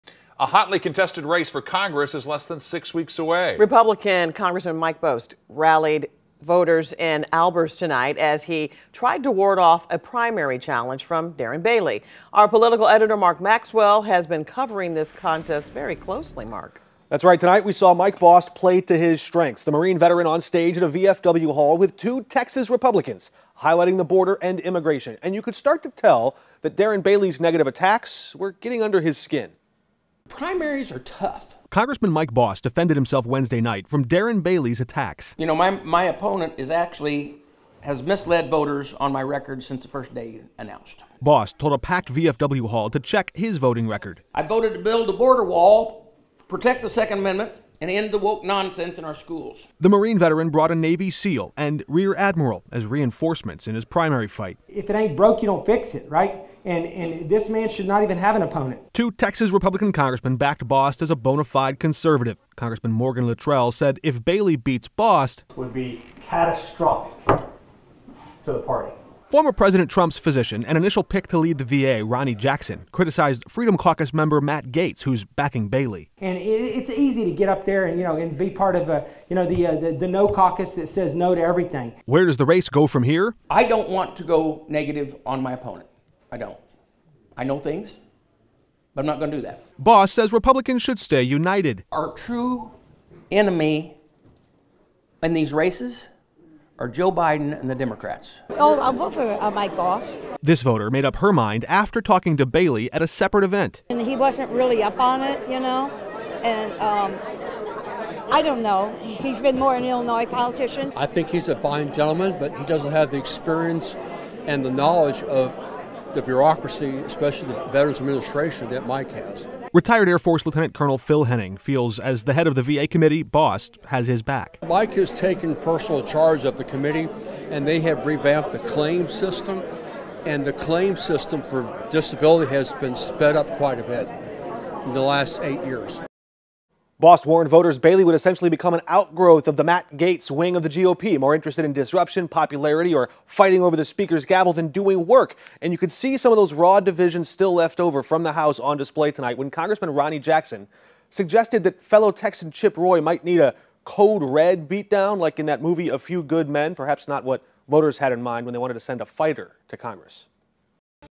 Texas Republicans rally with Bost in Illinois Congressional race
Mike Bost was on stage at a VFW Hall with two Texas Republicans, highlighting the border and immigration. You could tell Darren Bailey's negative attacks were starting to get under his skin.